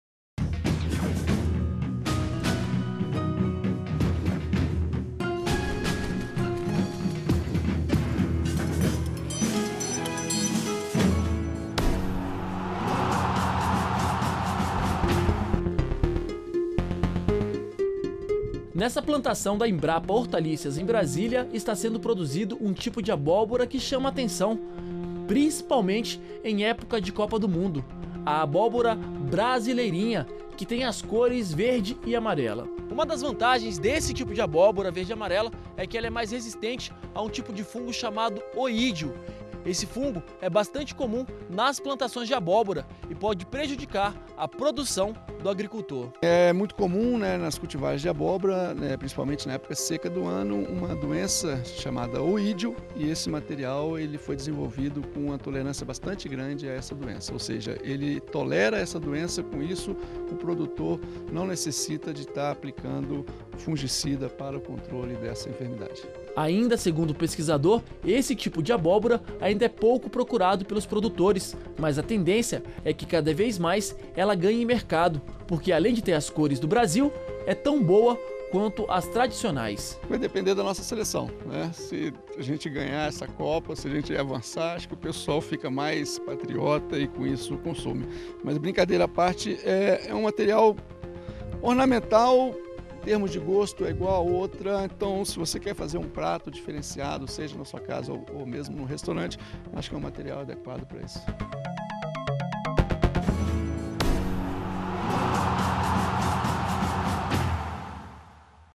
Embrapa trabalha em produção de abóbora resistente a fungos
BRASIL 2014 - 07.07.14: Uma nova abóbora produzida na sede da Embrapa Hortaliças, em Brasília, chama a atenção em tempos de Copa do Mundo. Nas cores verde e amarela, a abóbora brasileirinha, como é chamada, é mais resistente a um tipo de fungo comum nas plantações do alimento. A nova forma da hortaliça ainda é pouco procurada, mas deve ganhar o mercado em breve.